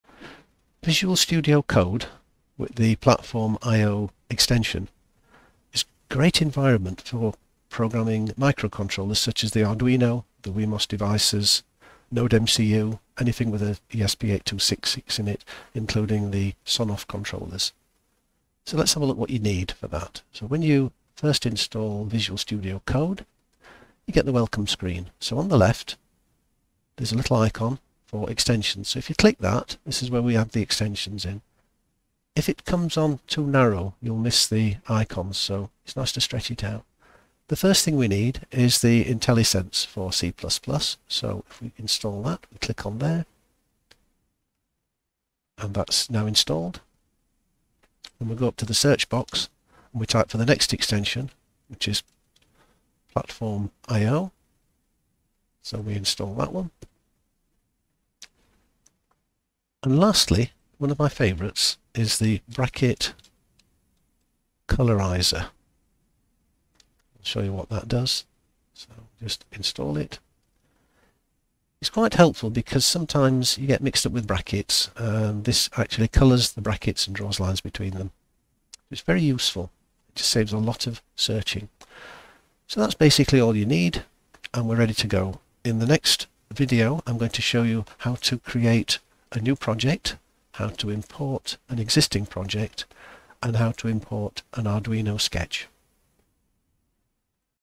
Preparing Visual Studio Code & PlatformIO for Arduino / ATtiny / ESP8266 / ESP32 / SonOff, etc.
We walk you through setting up Visual Studio Code with the extension add-on for PlatfomIO. An easy way to prepare for your first VSC Arduino coding experience.